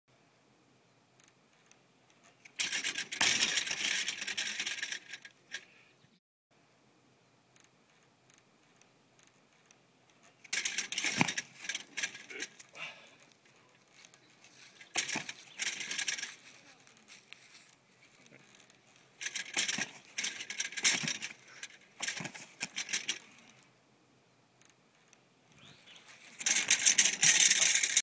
My younger brother and his slackline backflip
Yeah, we used to compete at this. Then he landed, what is said to be, the first backflip on a slackline landed in Sweden along with a bunch of other sweet tricks (some of them showed in this video), this is an old clip I found of it.